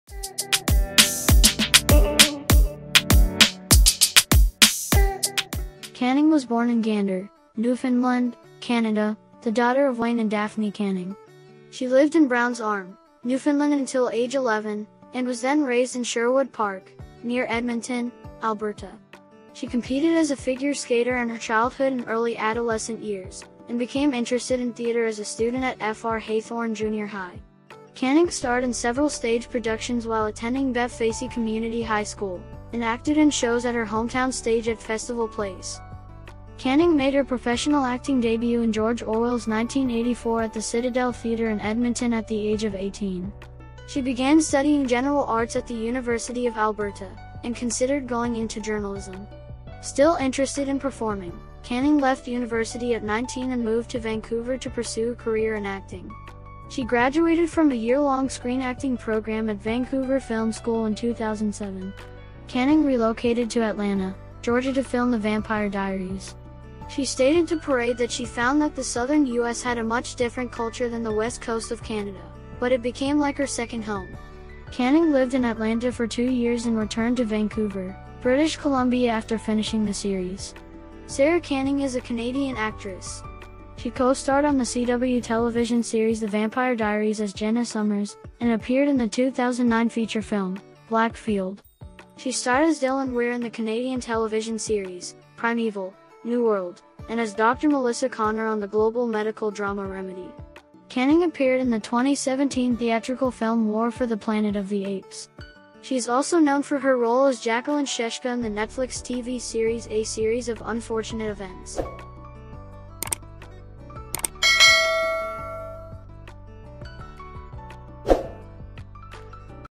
Who is Sara Canning?
Sara Canning is a Canadian actress. She co-starred on The CW television series The Vampire Diaries as Jenna Sommers, and appeared in the 2009 feature film, Black Field. She starred as Dylan Weir in the Canadian television series, Primeval: New World, and as Dr. Melissa Conner on the Global medical drama Remedy.
Musician: @iksonmusic